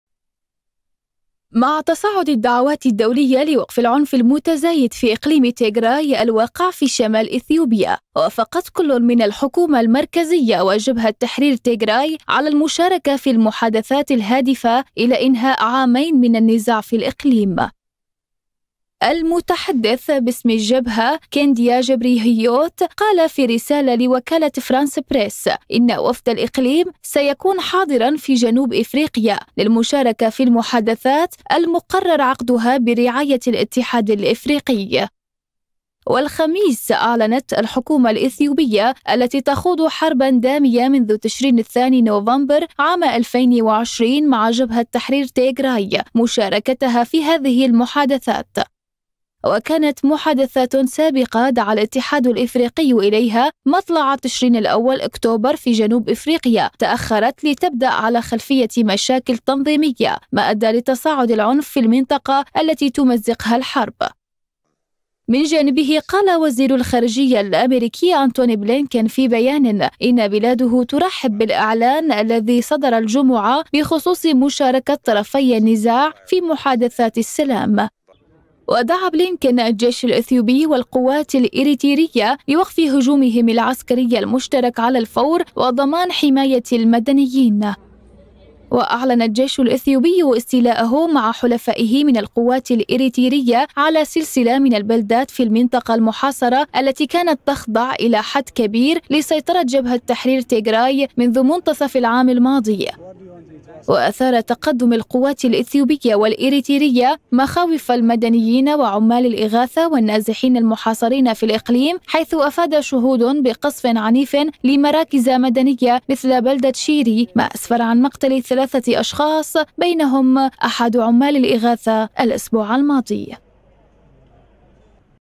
جبهة تيغراي تؤكد مشاركتها بمفاوضات السلام في جنوب إفريقيا
البث الحي / البث المباشر الأعلى مشاهدة

تابعوا قناة اليوم على التردد:
12687_27500 (H) _NILESAT